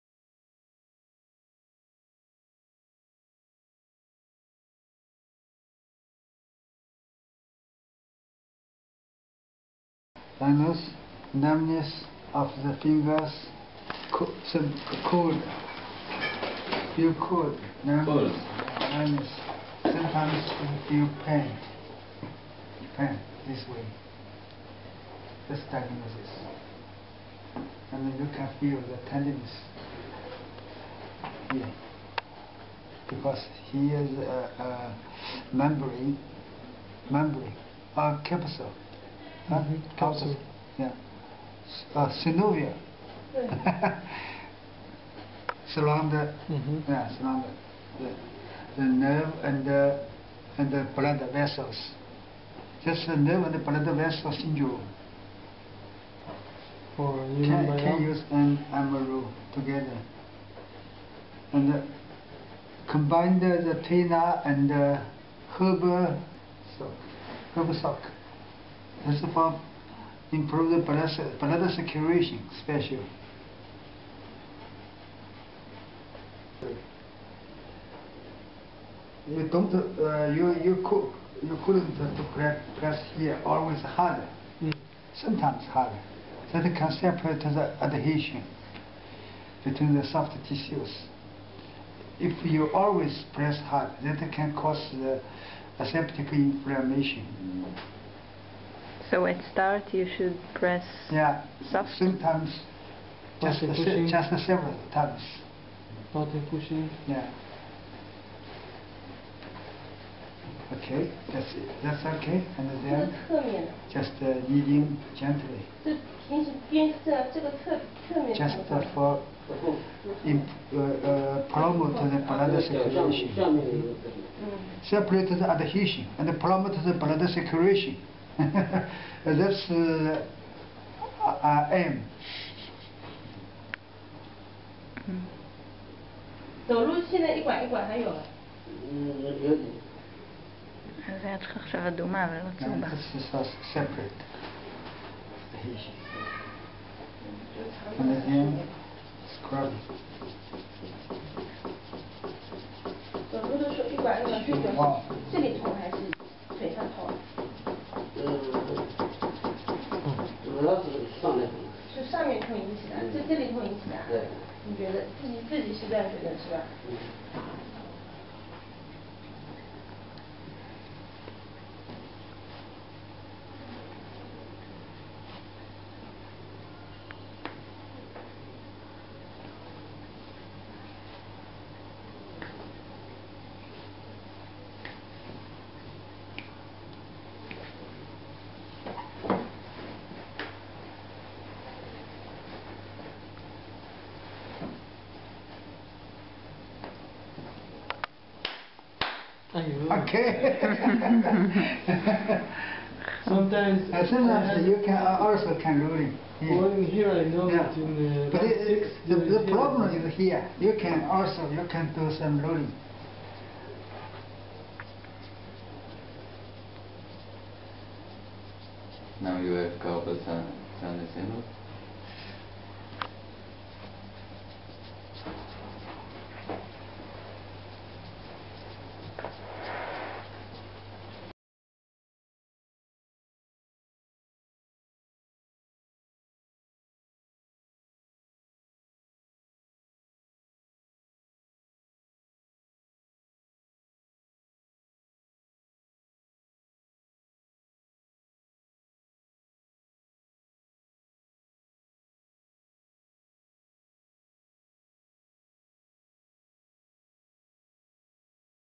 Carpal tunnel syndrome - Tuina treatment. טיפול טווינה תסמונת התעלה הקרפלית
Dr.  Li Hang demonstrate a Tuina treatment for Carpal Tunnel Syndrome.  Took place in
Zhejiang provincial TCM hospital, Hangzhou, Xiasha, China.  省中醫院，下沙， 浙江杭州,2009.